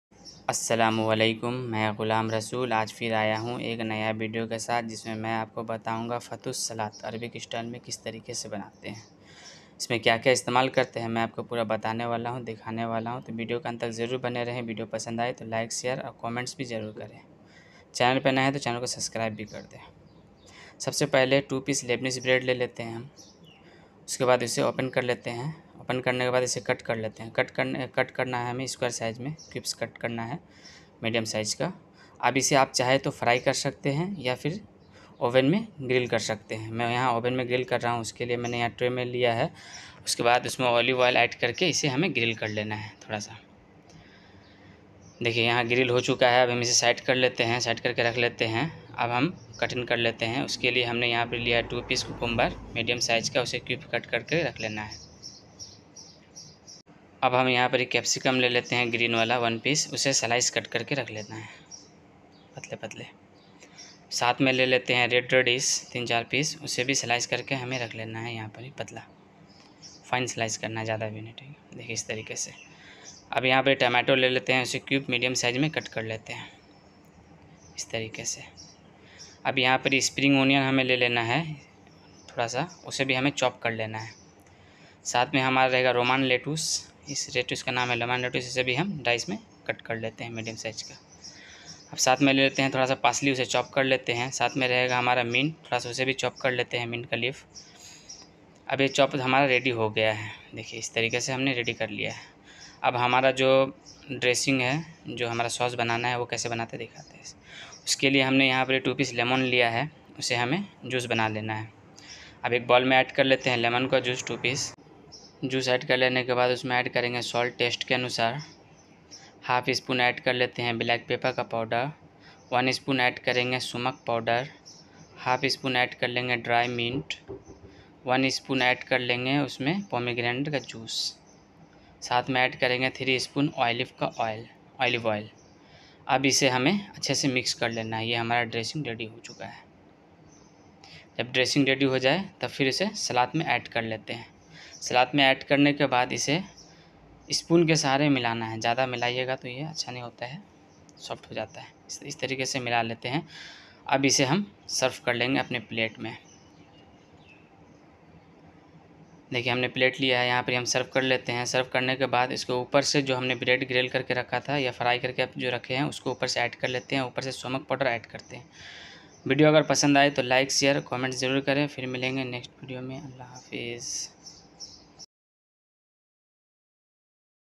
Fattoush salad recipe/ Fattoush salad Arabic/fattoush salad dressing/
fattoush salad recipe Fattoush salad Arabic

fattoush salad dressing

ingredients.

2 pes Lebanese bread

2 pes cucumber Cubs

4 pes red radish slice

2 pes green capsicum slice

1 pes tomato cubs

little spring onion

little parsley chopped

little mint chopped

2 pes lemon juice

half spoon black papper powder

1 spoon sumac powder

half spoon dry mint

2 spoon pomegranate juice/molasses